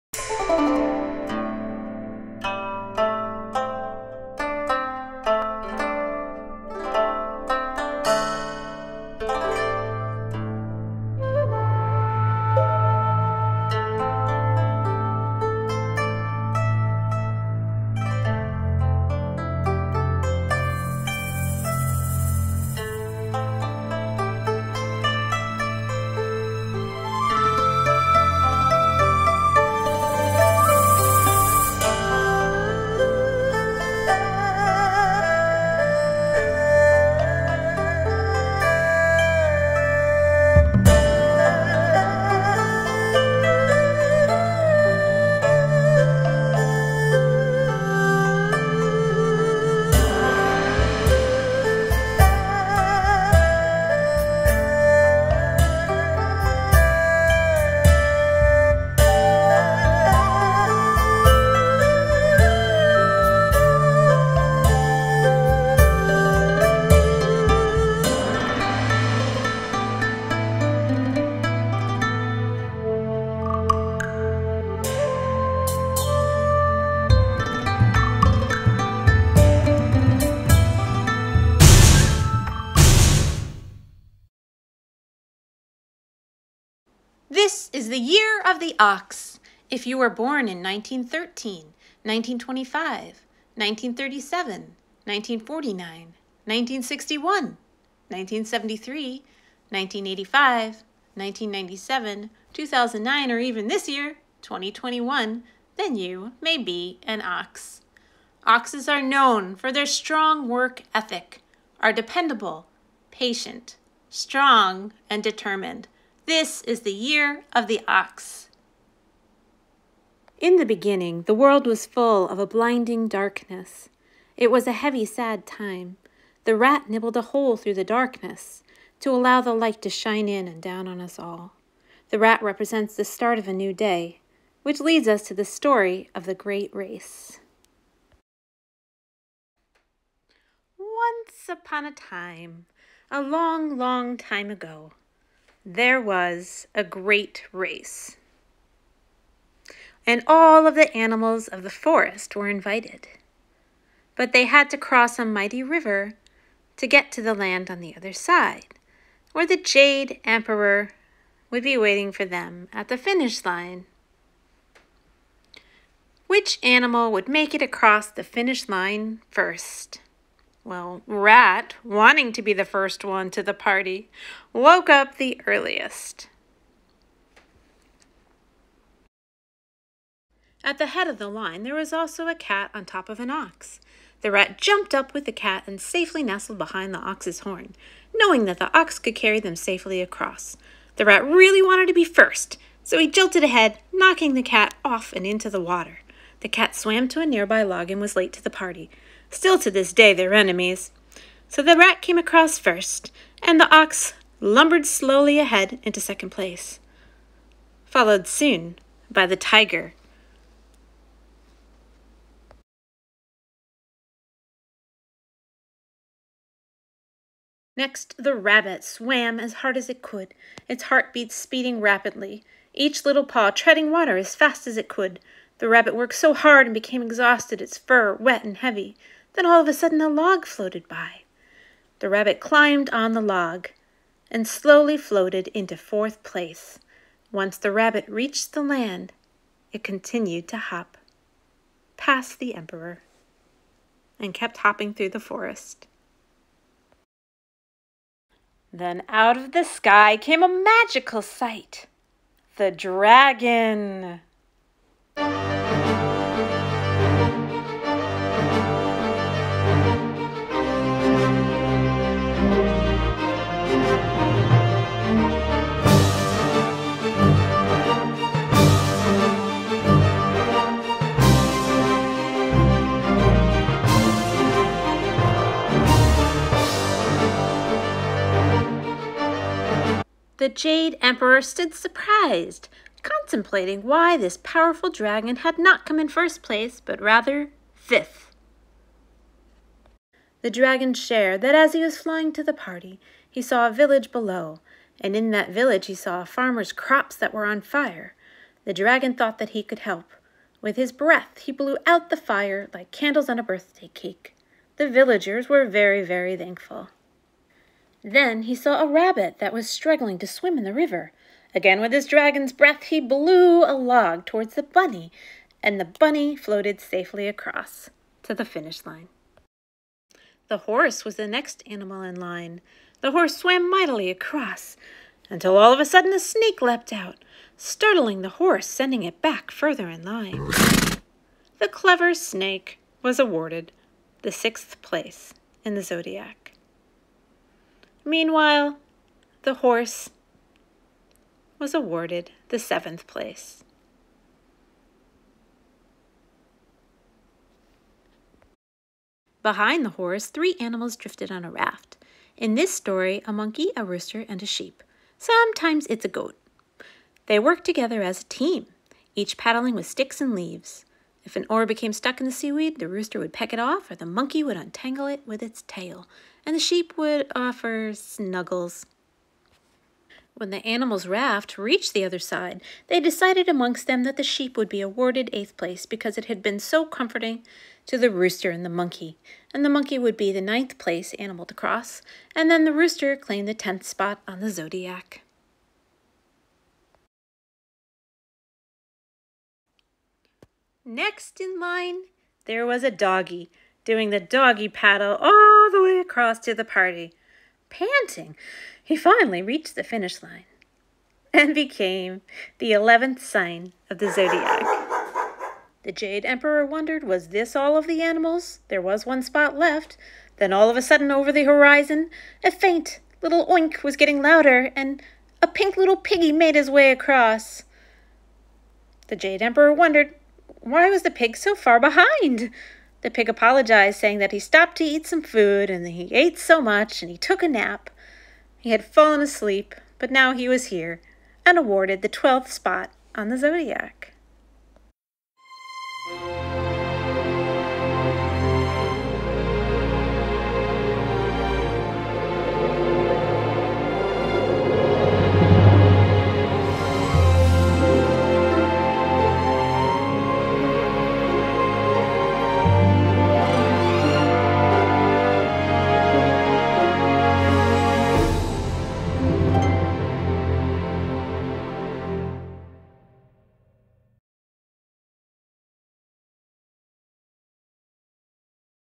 Year of the Ox and The Great Race, a stop motion shadow puppet animation by Kimberly Van Aelst
Kimberly Van Aelst of the OompaPossum Puppets, an internationally award winning puppetry company, presents The Ox and the Great Race, featuring handcrafted puppets. This stop-motion animation highlights the importance of the Ox in the legend of “The Great Race of the Animals” in the Chinese zodiac.
Watch Kim’s Ox Shadow Puppet workshop to learn how she created her shadow puppets and how you can create your own versions at home.